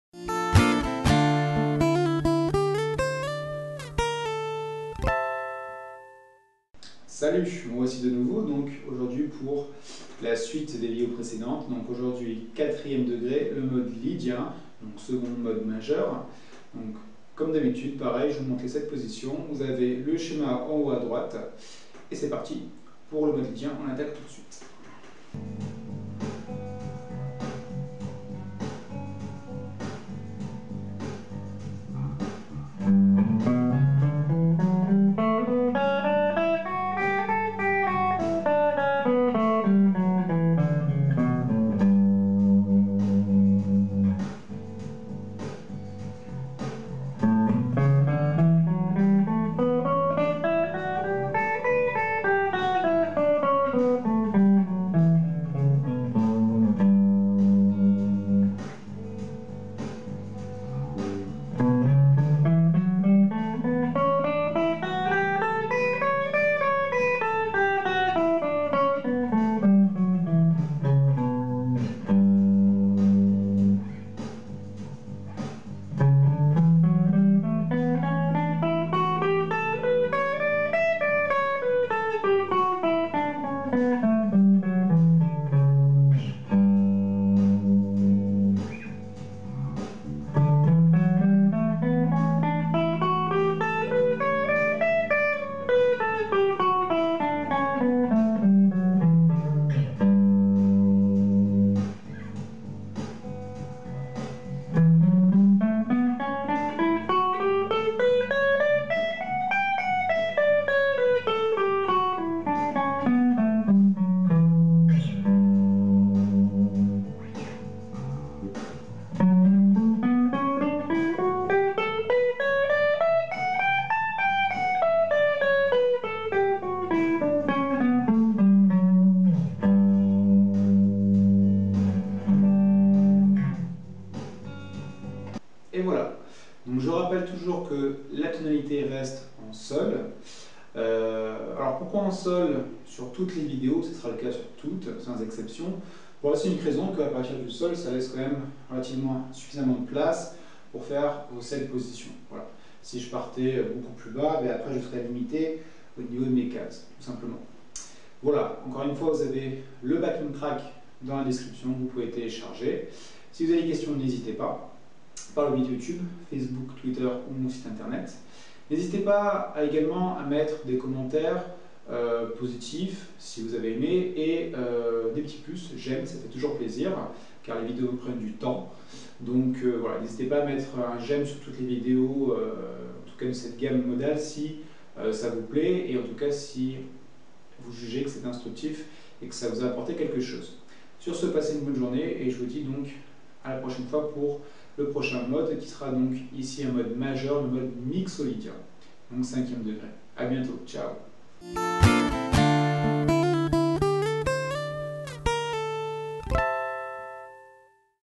Comment jouer les 7 Positions du Mode Lydien
Apprentissage des 7 positions du mode Lydien avec schéma !

Bonne apprentissage !